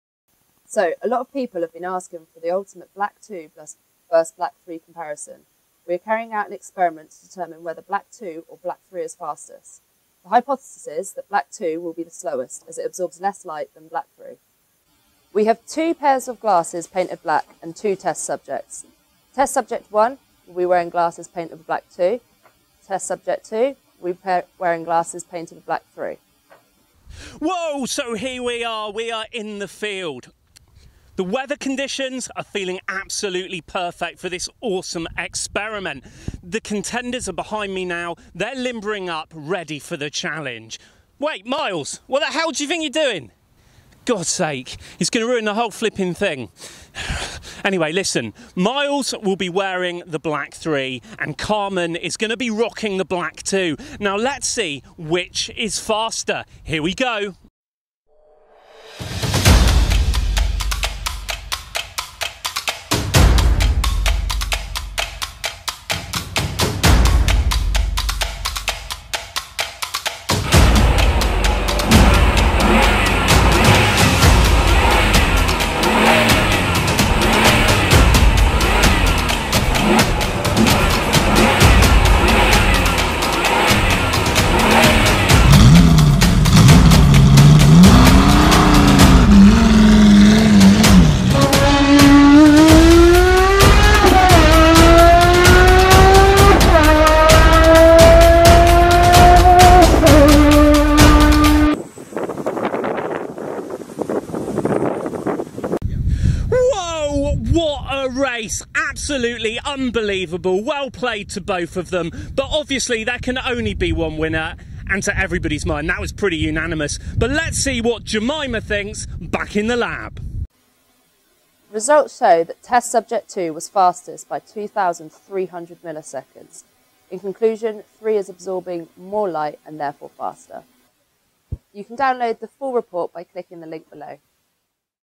Black 2.0 vs Black 3.0 - which is faster?
We put black 2.0 and Black 3.0 to the ultimate paint test! A lot of you have been asking and we wanted to be sure - which one is fastest!

We don't have any black 3.0 to share but we will at some point in the future, and if you want some it's probably a good idea to add yourself to the waitlist here:

Here's jemima's full study!